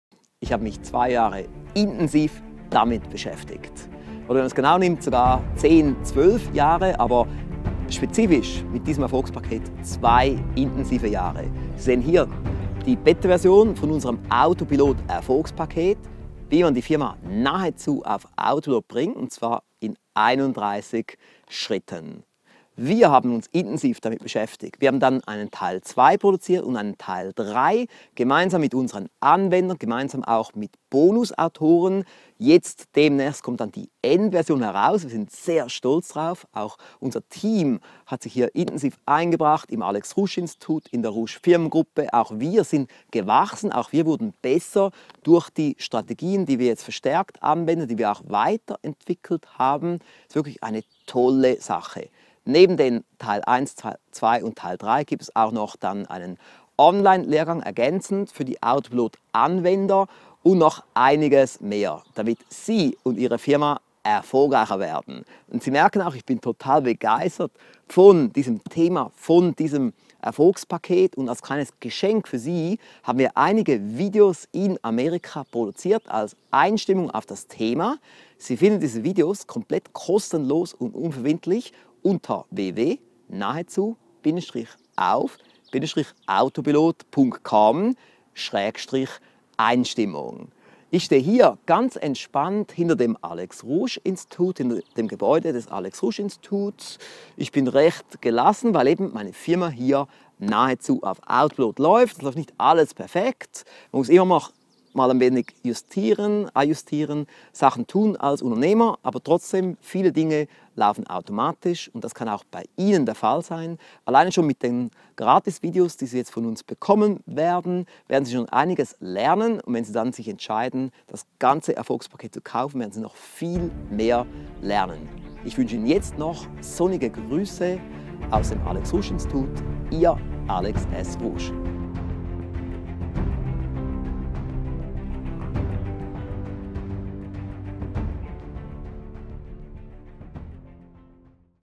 Alex S. Rusch präsentiert Gratis-Videos als Einstimmung zum Thema "Die Firma auf Autopilot bringen".
- lernen Sie von Alex S. Rusch wie Sie Ihre Firma nahezu auf Autopilot bringen.